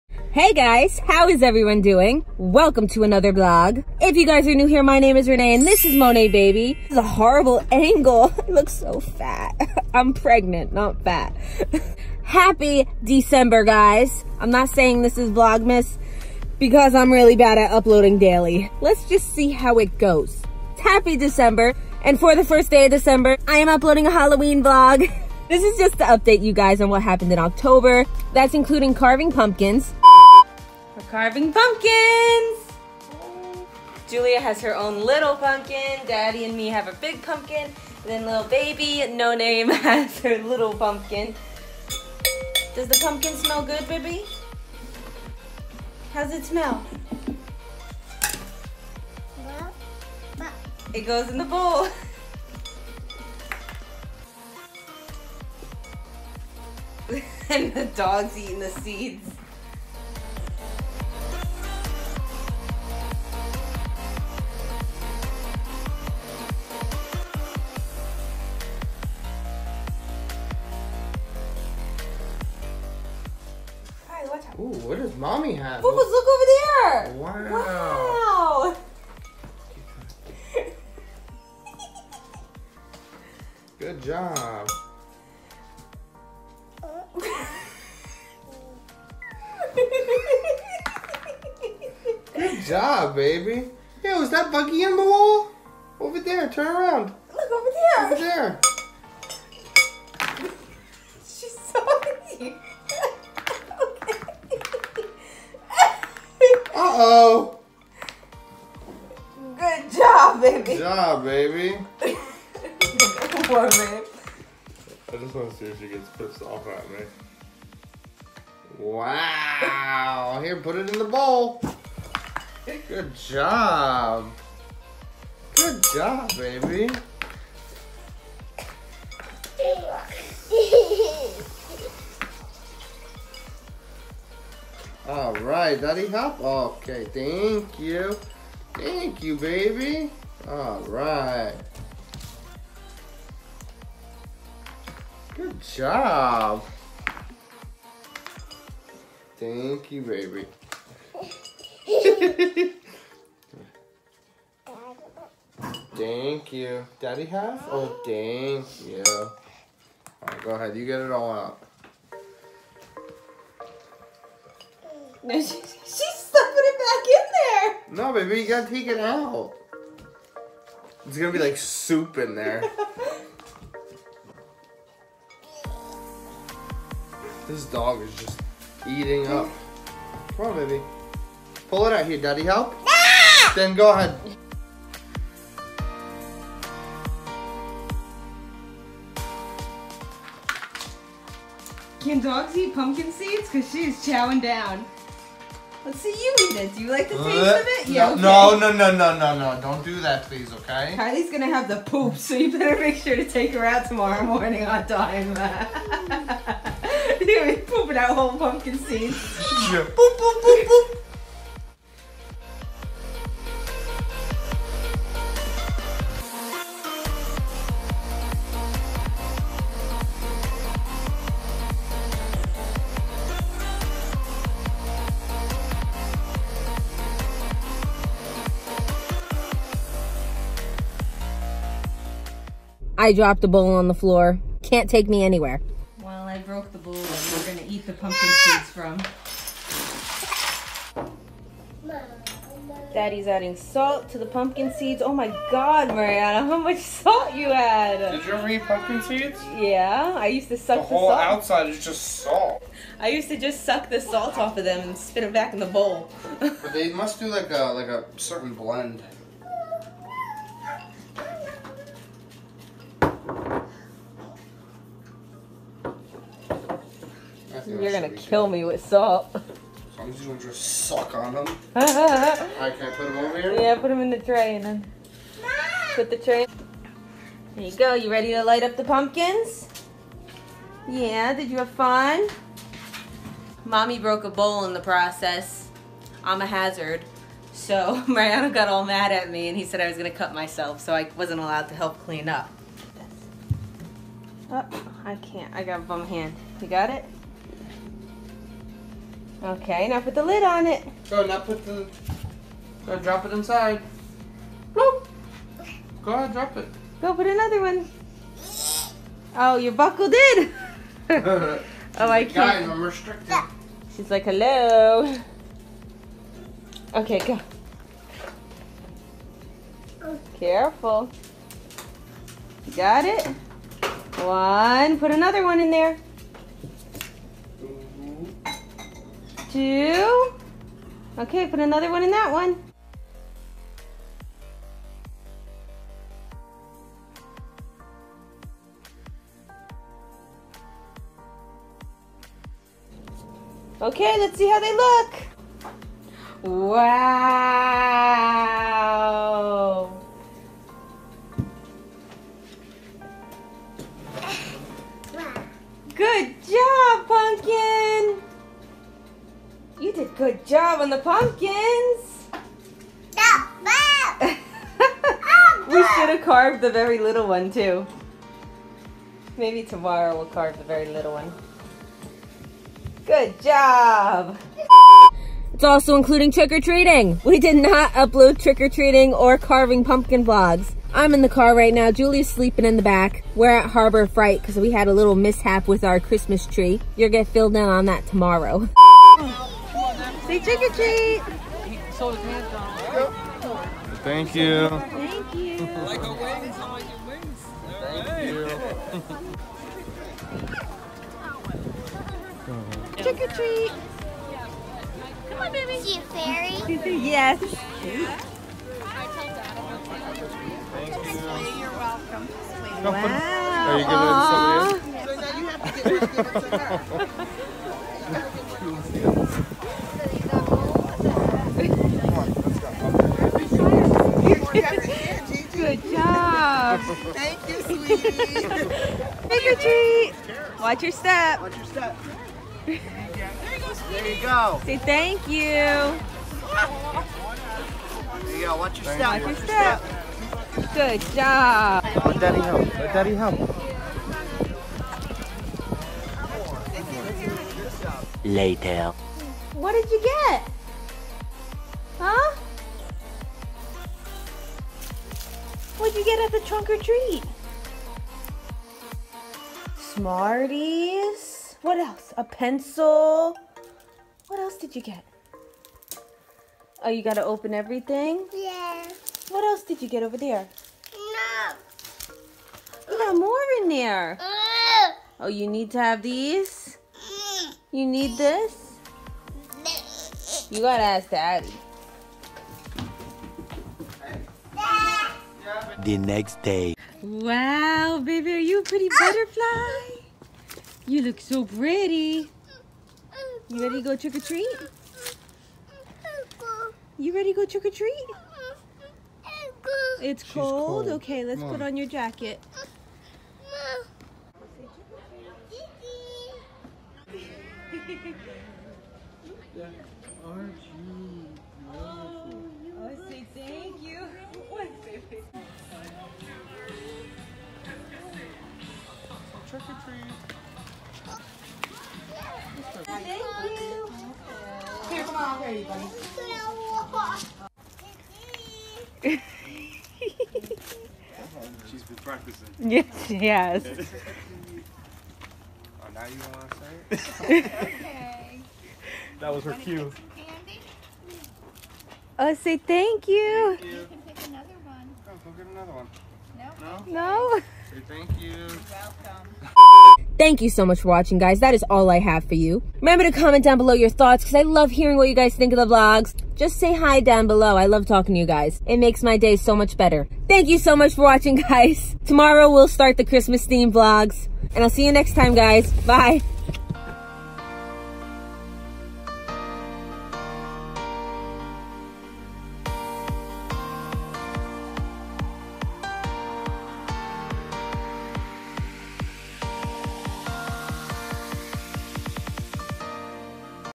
THE NIGHTMARE BEFORE CHRISTMAS!
Halloween has taken over the first episode of  Vlogmas!!

🤰🏼P.S I’m 34 weeks and 2 days pregnant now!🤰🏼